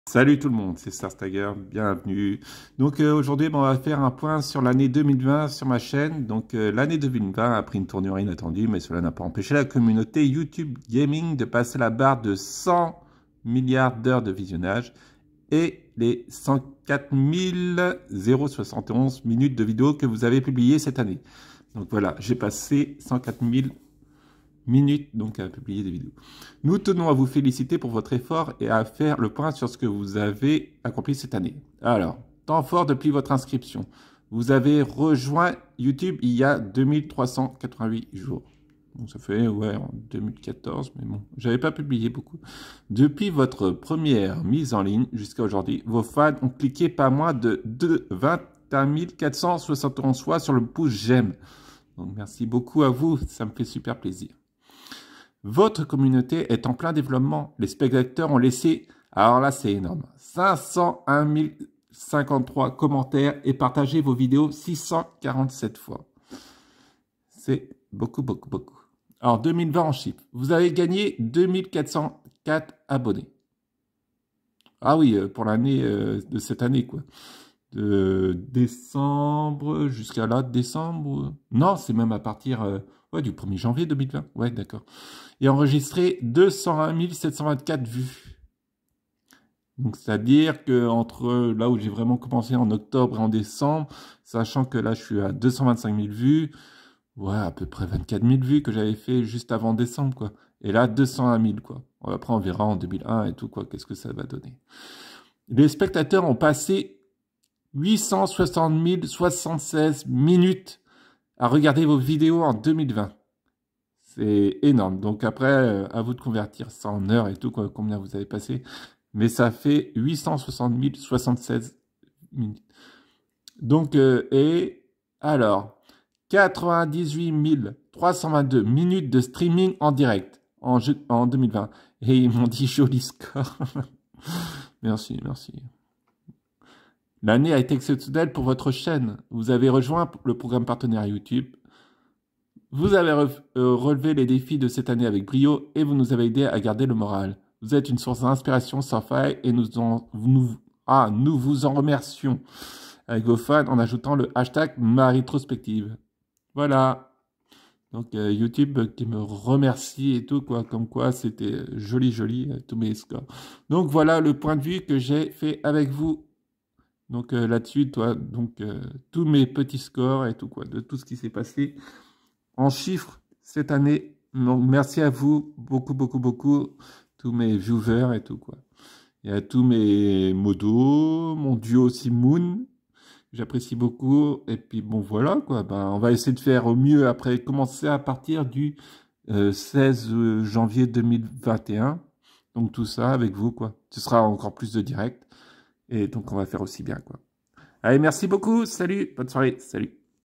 Toutes mes Statistiques Chiffres de youtube année 2020 et c'est énorme ! merci à tous !
je vous montre toutes mes statistiques du 1 er janvier 2020 jusqu au 10 décembre
les chiffres sont énormes merci  c'est grace à vous , je recommencerai les lives dés le 16 janvier .
Merci à vous les viewers , les modos et à ma duo moon dessin